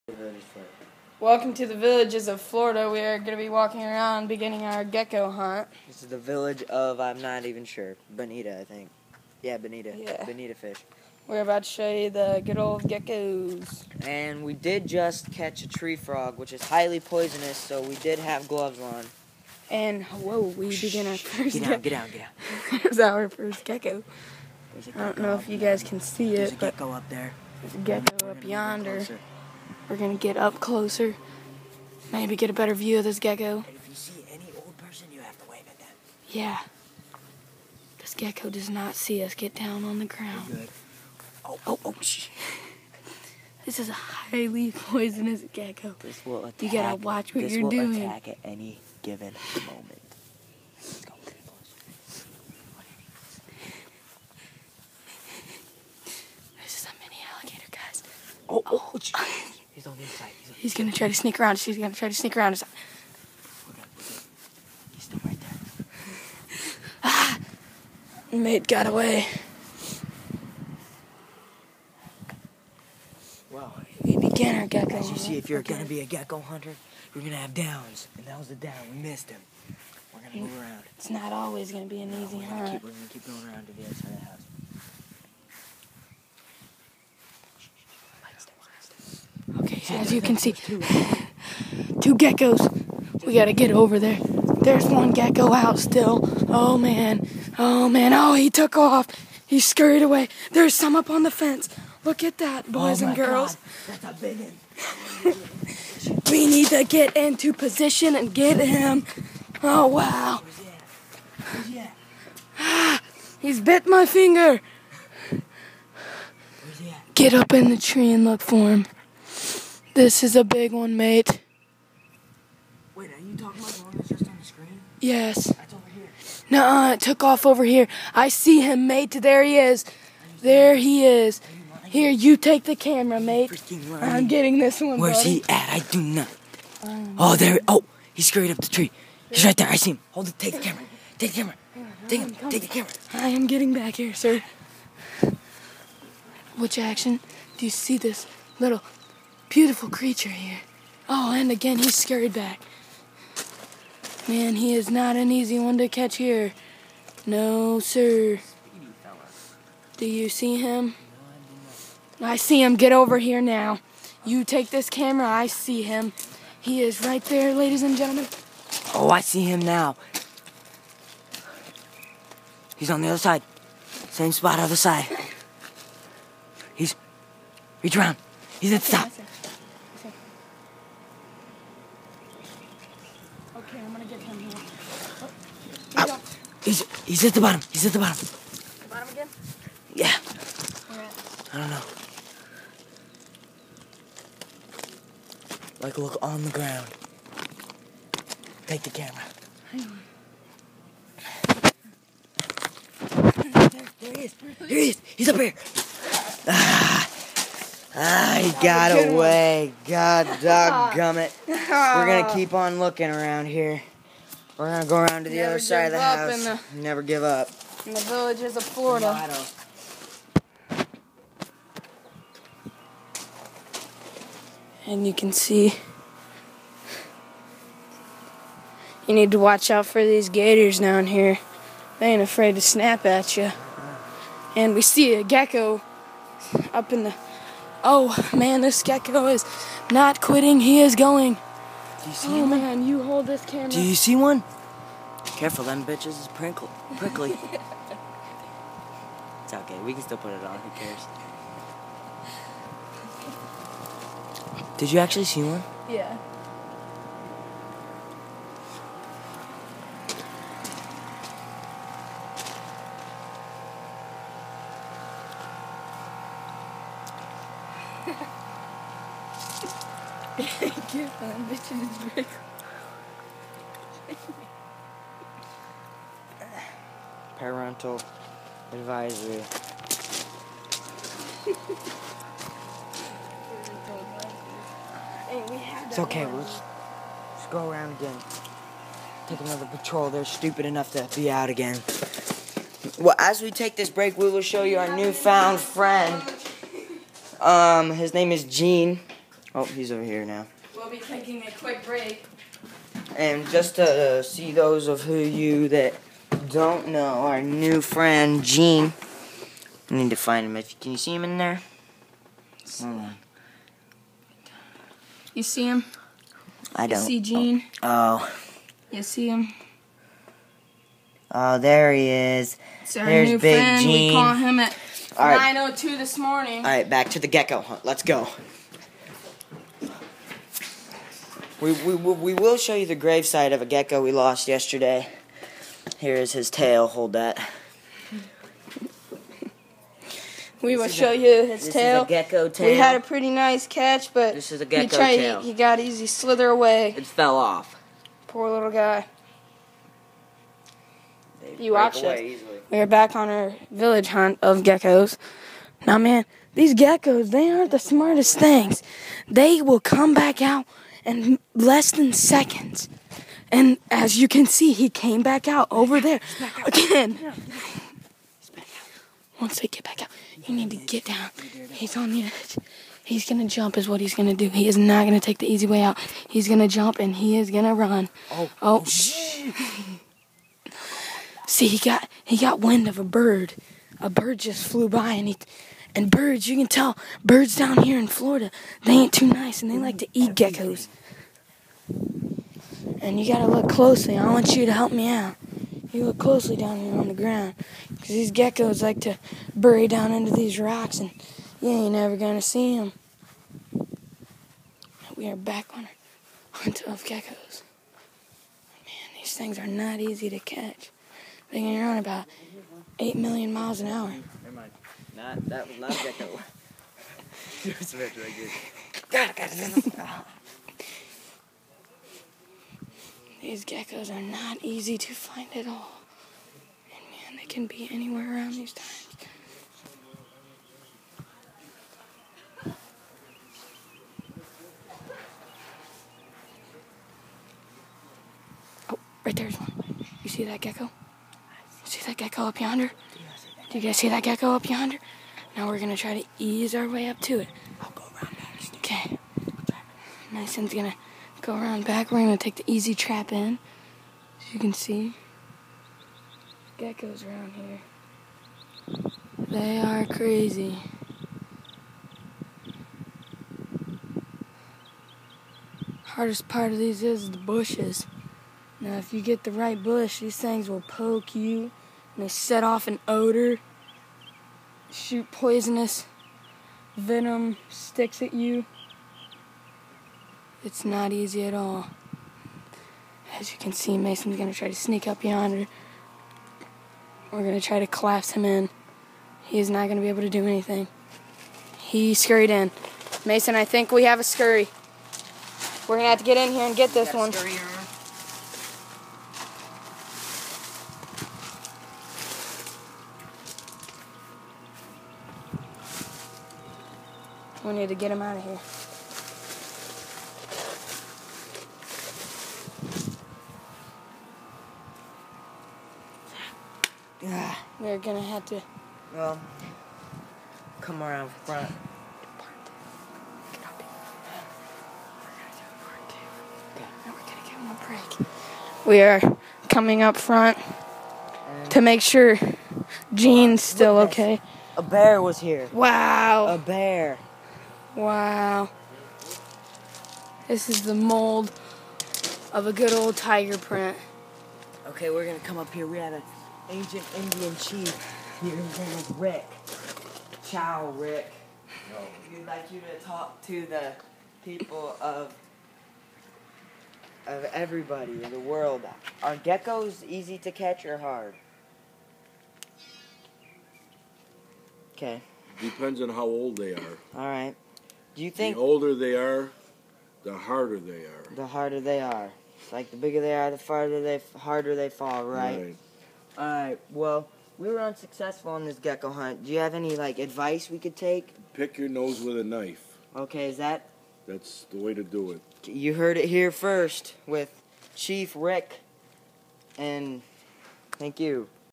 Gecko hunters in Florida
Me and my buddy hunting geckos